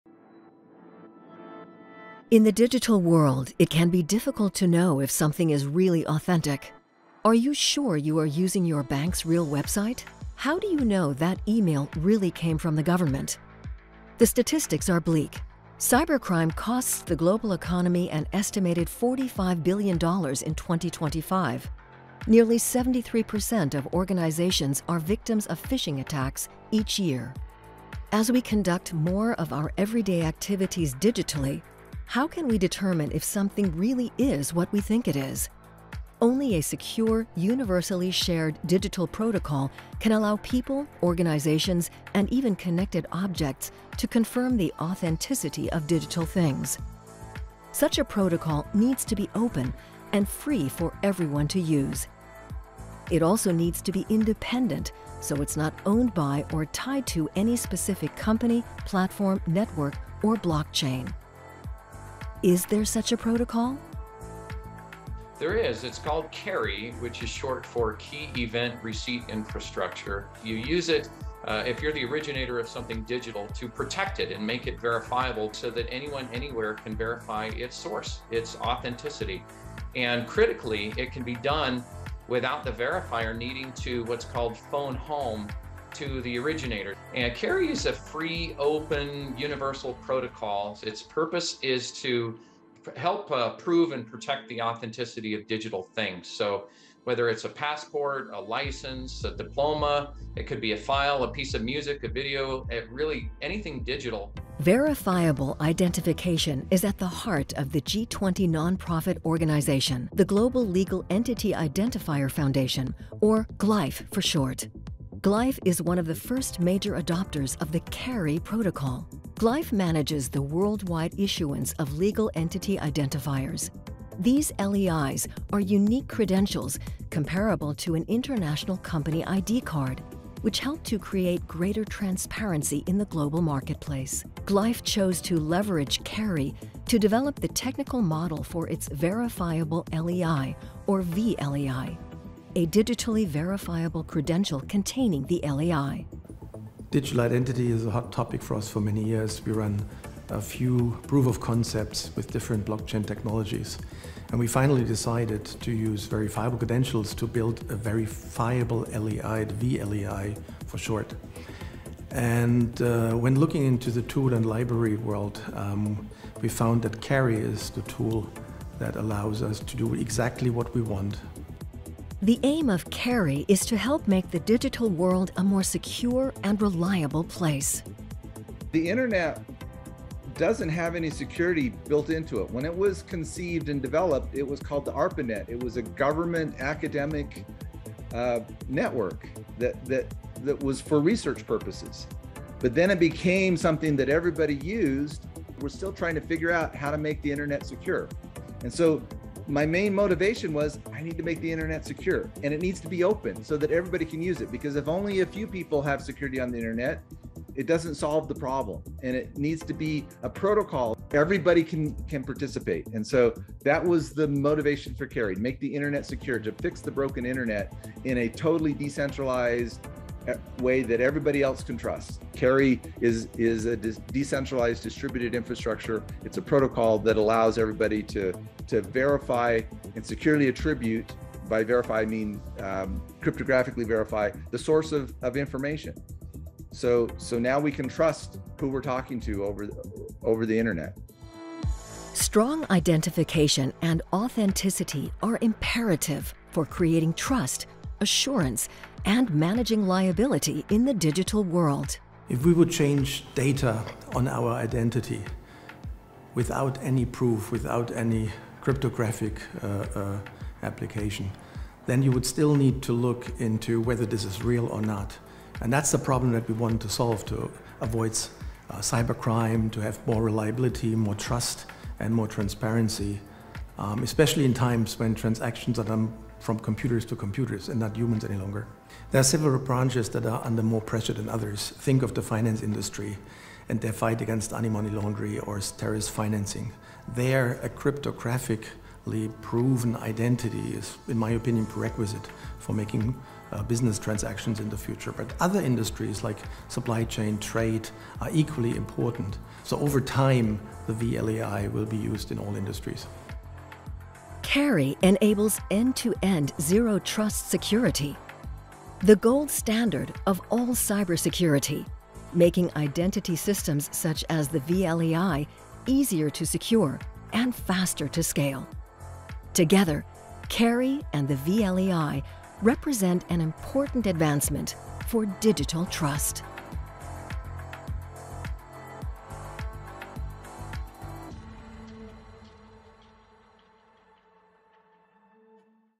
Digital Identity: It’s All About Authenticity
How can you be certain something is real in the digital world? In the absence of a handshake or physical signature, confidence in digital authenticity is in short supply. How can you be sure you are using your bank’s real website? Did that email really come from your business partner?

As a result, there is an urgent unmet need for greater security, convenience, and ease-of-use in digital identity for organizations and the persons that represent them in either official or functional roles. Specifically, advances are needed in the fields of strong authentication, identity verification, and proof of data authenticity.

Through the creation of the verifiable Legal Entity Identifier (vLEI), GLEIF has created a universal system of organizational identity that answers this need. The vLEI enables instant and automated business verification for all legal entities and their official representatives.

The vLEI leverages the Key Event Receipt Infrastructure (KERI) protocol to adopt a ‘network of networks’ approach that enables seamless and secure interoperability with all technology models, including blockchain, cloud services, and APIs.

Find out more about KERI and vLEI in the following video and on this blog